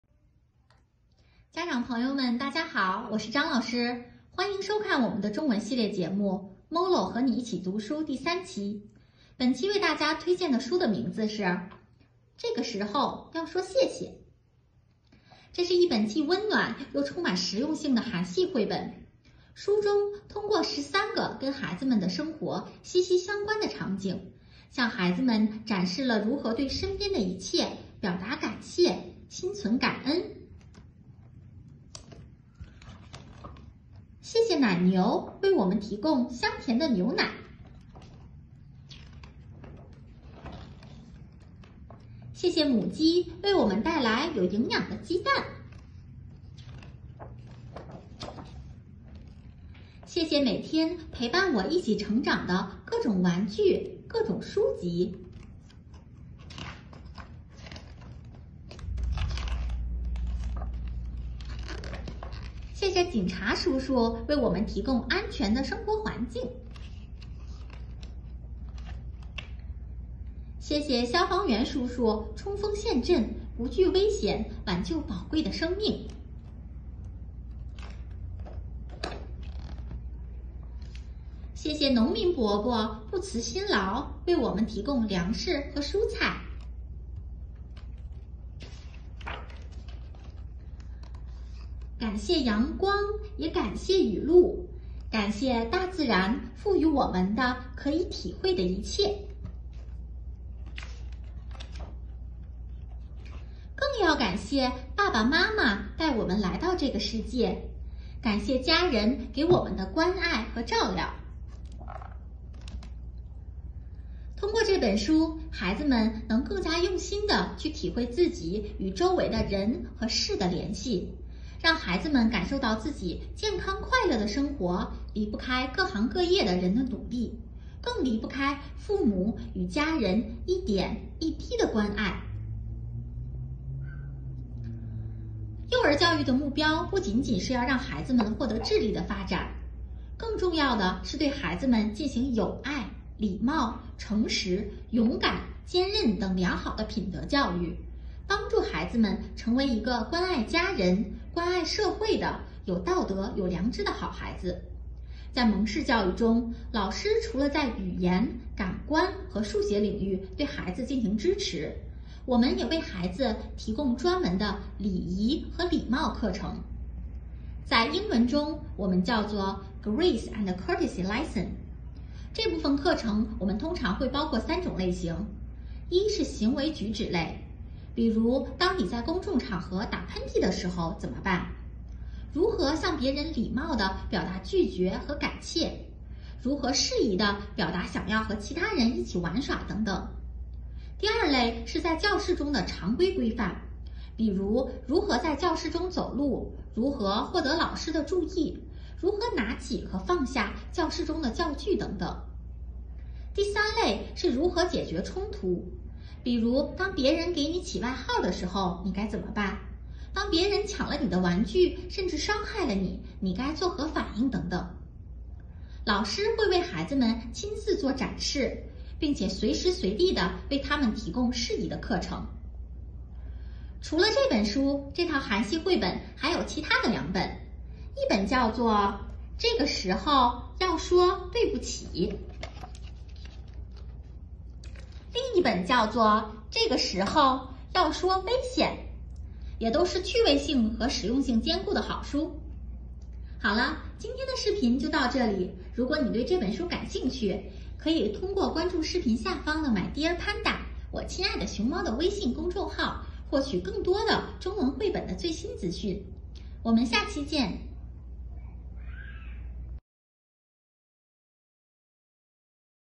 MOLO和你一起读书03期
家长朋友们，大家好，我是张老师，欢迎收看我们的中文系列节目，MOLO和你一起读书第三期，本期为大家推荐的书的名字是《这个时候要说“谢谢”》。这是一本既温暖，又充满实用性的韩系绘本，书中通过十三个跟孩子们的生活息息相关的场景，向孩子们展示了如何对身边的一切表达感谢，心存感恩。如果你对这本书感兴趣，可以通过关注视频后的My Dear Panda 我亲爱的熊猫的微信公众号获取更多的中文绘本的最新资讯。